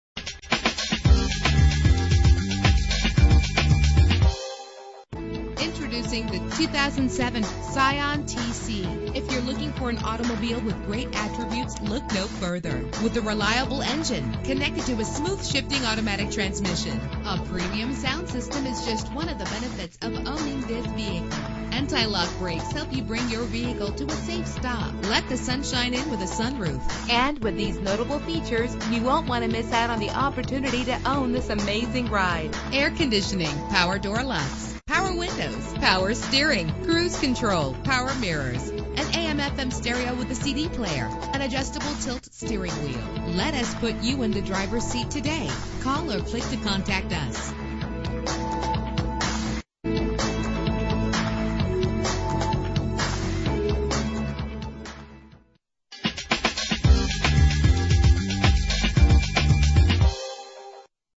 2007 Scion tC - Chandler AZ
Year: 2007
Make: Scion
Model: tC
Trim: 2D Coupe
Engine: 2.4 liter I-4
Transmission: Automatic
Color: Blue
Mileage: 103745
Address:
7300 West Orchid Lane